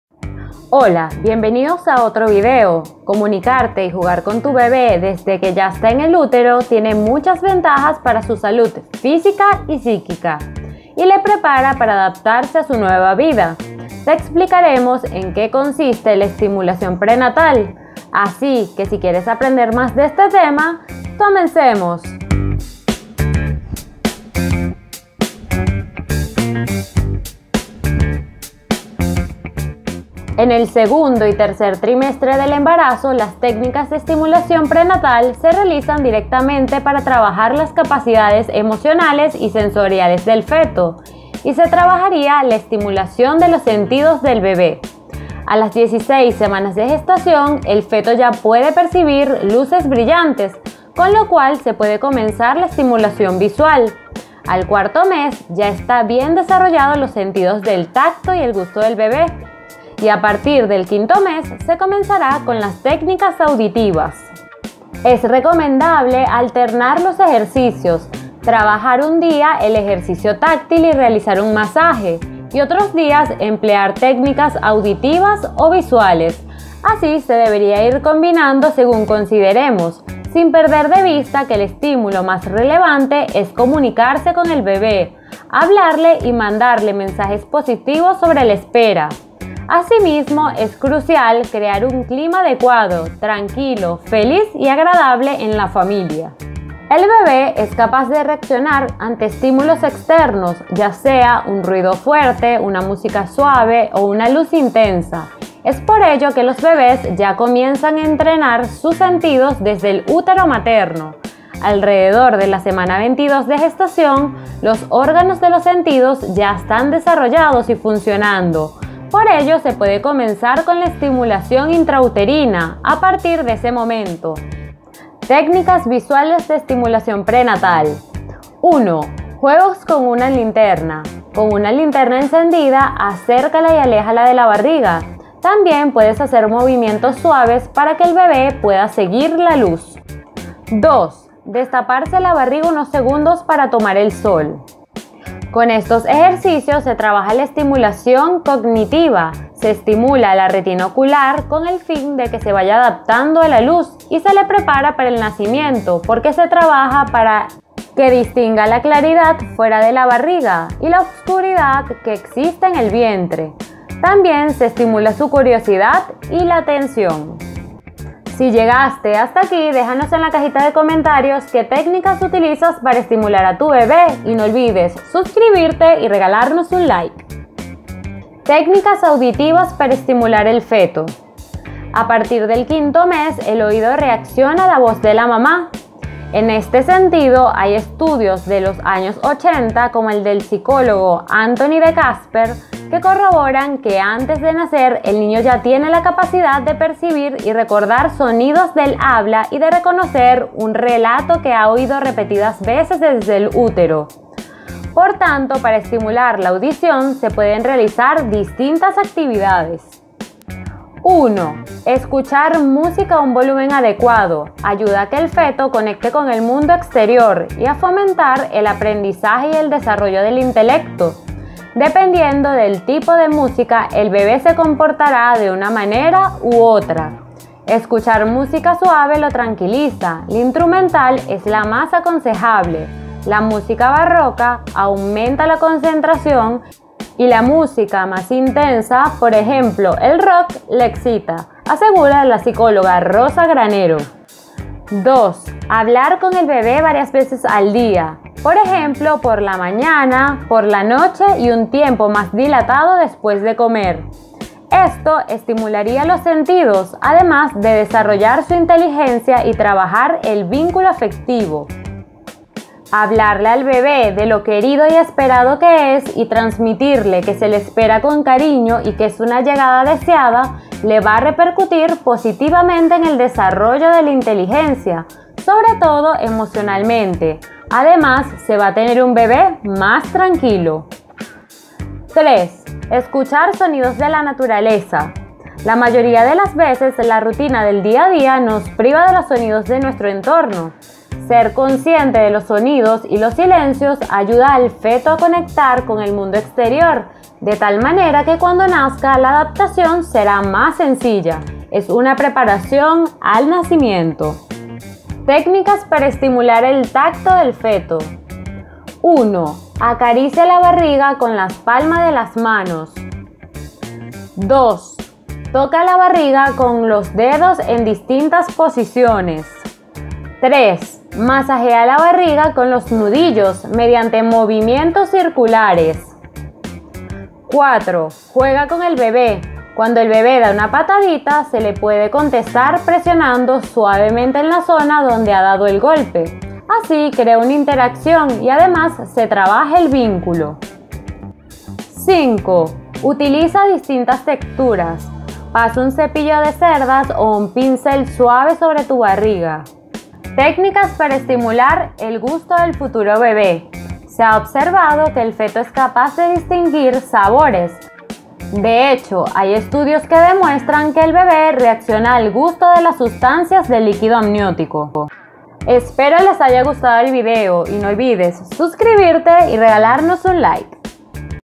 ESTIMULACIÓN PRENATAL : 🎵👶🏻 CÓMO ESTIMULAR AL BEBÉ EN EL VIENTRE
- Cómo estimular al bebé durante el embarazo
- Estimulación prenatal del bebé
- Técnicas para estimular al bebé en el vientre

Estimulación prenatal
Comunicarte y jugar con tu bebé desde que está en el útero tiene muchas ventajas para su salud física y psíquica, y le prepara para adaptarse a su nueva vida. Te explicamos en qué consiste la estimulación prenatal.

En el segundo y tercer trimestre del embarazo las técnicas de estimulación prenatal se realizan directamente para trabajar las capacidades emocionales y sensoriales del feto, y se trabajaría la estimulación de los sentidos del bebé. A las 16 semanas de gestación, el feto ya puede percibir luces brillantes, con lo cual se puede comenzar la estimulación visual. Al cuarto mes ya están bien desarrollados los sentidos de tacto y gusto del bebé. Y, a partir del quinto mes, se comenzará con las técnicas auditivas.

Lo recomendable es alternar los ejercicios. Trabajar un día algún ejercicio táctil y realizar un masaje, y otros días emplear técnicas auditivas o visuales; así se debería ir combinando según consideremos, sin perder de vista que “el estímulo más relevante es comunicarse con el bebé, hablarle y mandarle mensajes positivos sobre la espera. Asimismo, es crucial crear un clima adecuado, tranquilo, feliz y agradable en la familia”.